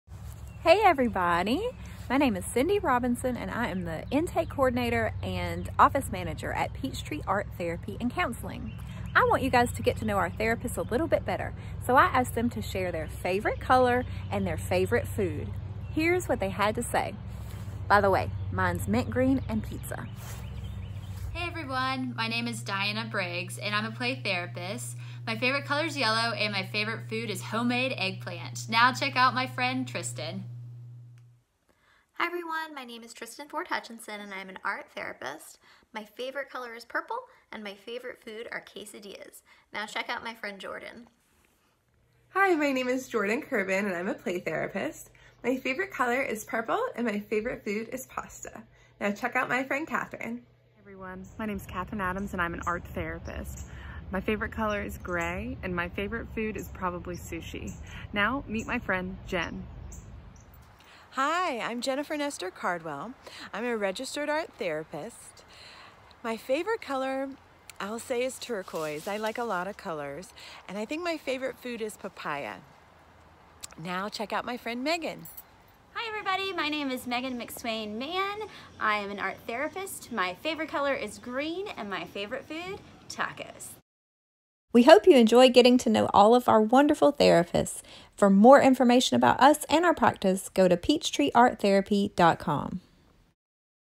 PATC Staff Inside Scoop
Our therapists are sharing their favorite colors and food in this inside-scoop video! We love and miss our team!